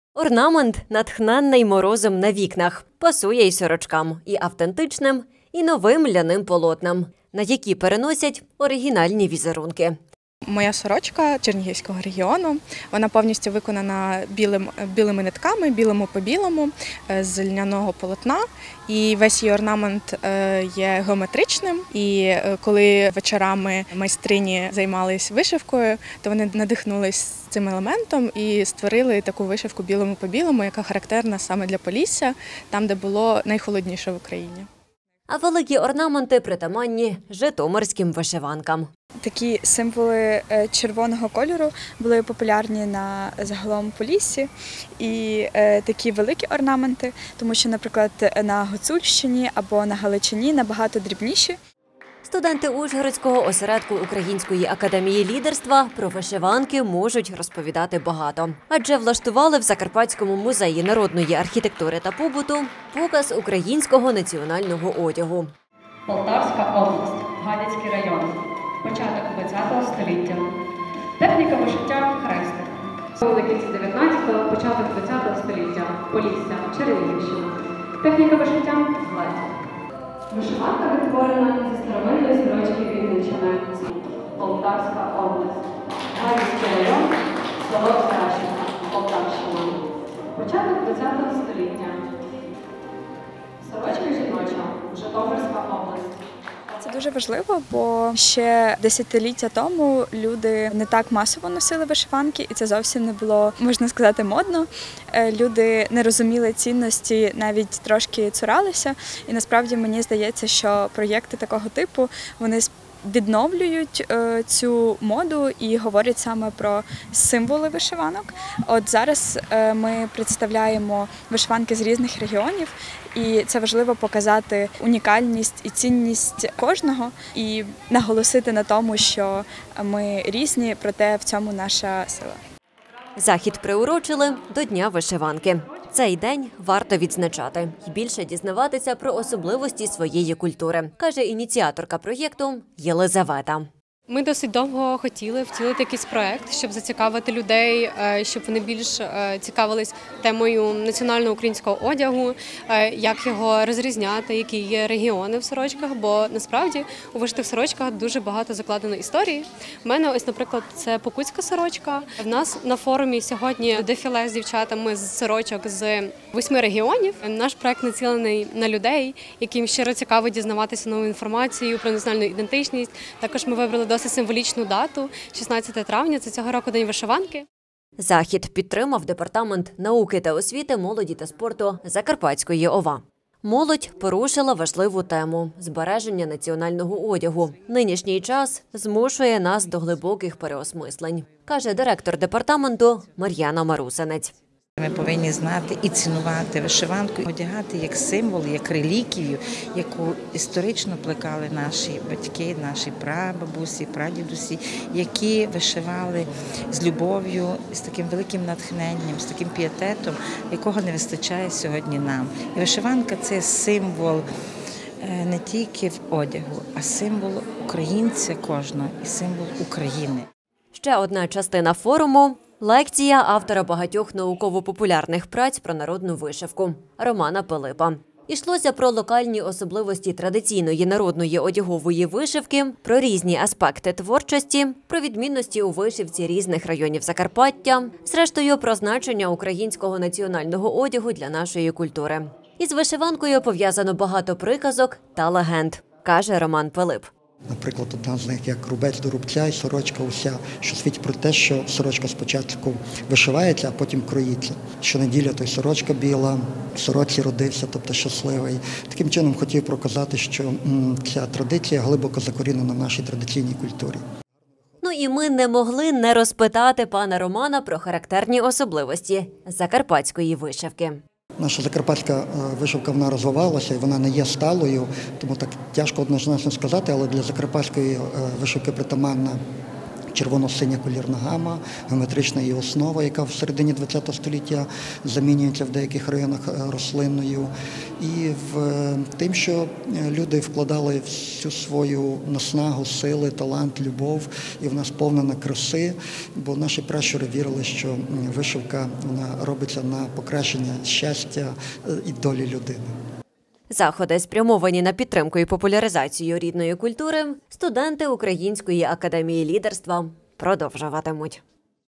Форум-виставка українського національного одягу до Дня вишиванки
Студентки ужгородського осередку Української академії лідерства влаштували в Закарпатському музеї народної архітектури та побуту показ українського національного одягу. Ще одна частина форуму – лекція  автора багатьох науково-популярних праць про народну вишивку Романа Пилипа.